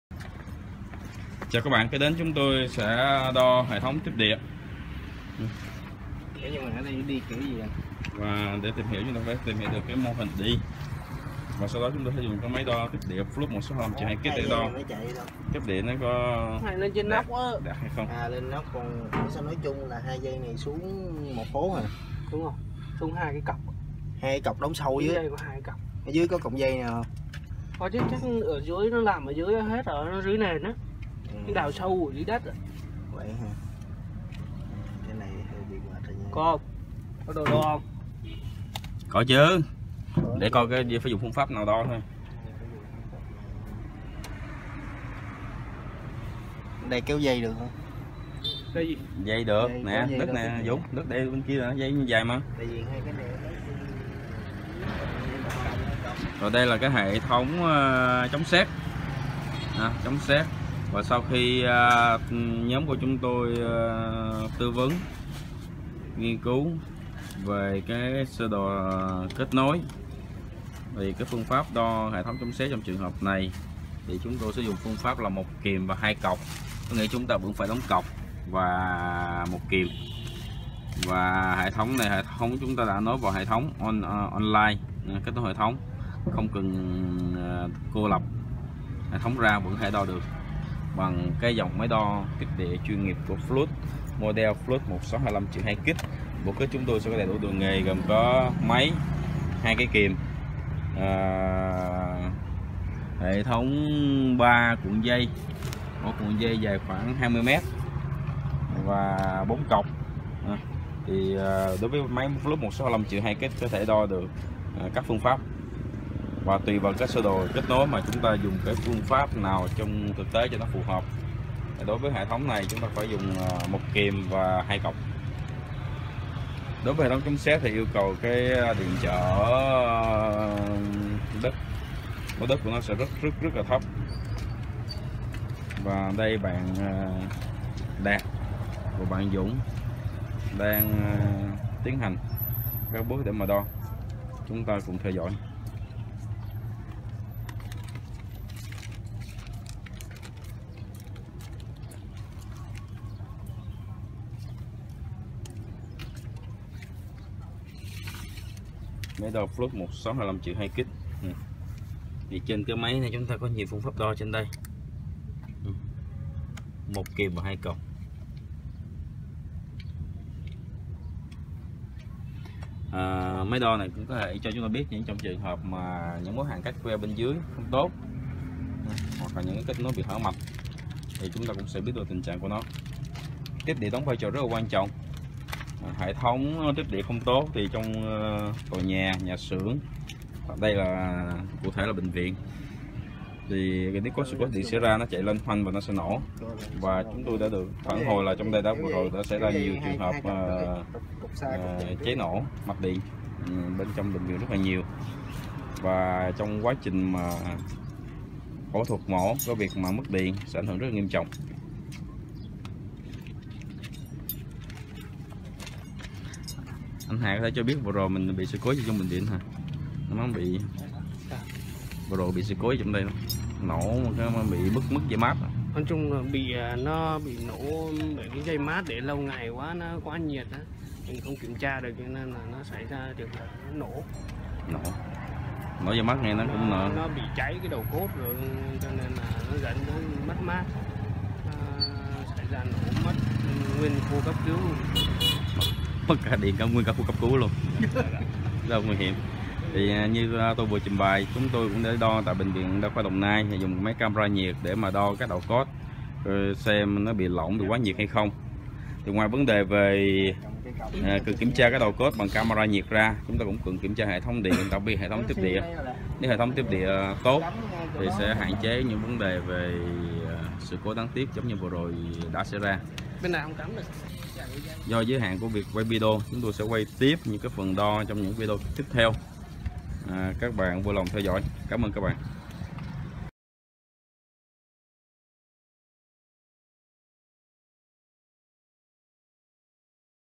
Đo tiếp địa chống sét với FLUKE-1625-2 KIT - P1
Thực hành đo tiếp địa chống sét tại Bệnh viện Đồng Nai với máy đo điện trở đất FLUKE-1625-2 KIT.
Phần 1: Nghiên cứu địa hình và lựa chọn phương pháp đo
Thực hiện bởi Fluke Việt Nam và công ty Unitek với sự hỗ trợ của Bệnh viện Đồng Nai
VPĐD FLUKE SOUTH EAST ASIA Pte Ltd tại TpHCM
Đ/c: 35 Tôn Đức Thắng, Quận 1, TpHCM